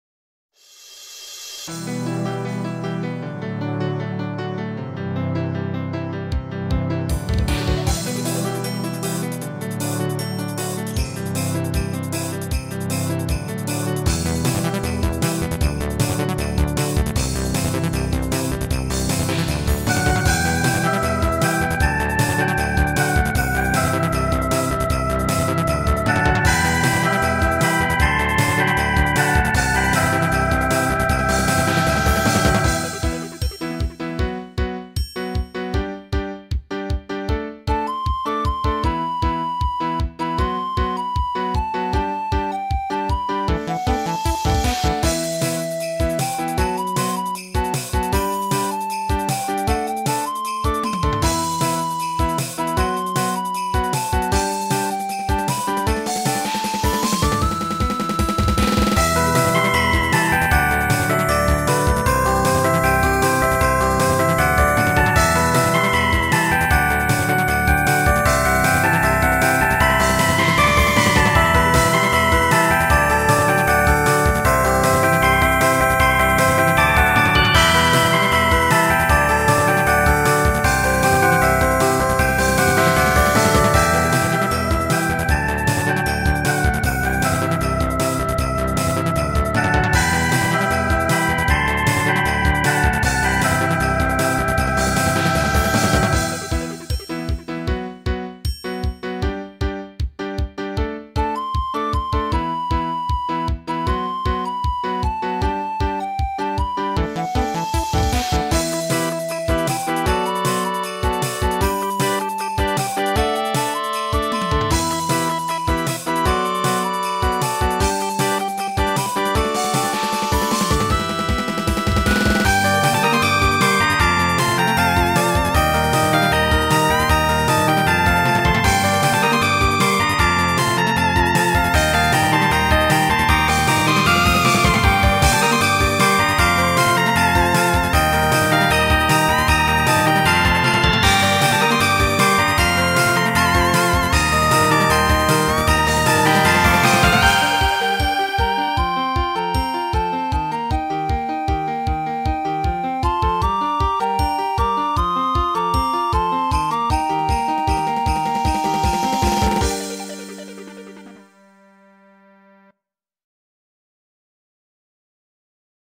I made a song using only Windows General MIDI sounds
MSGS is very fun to work with.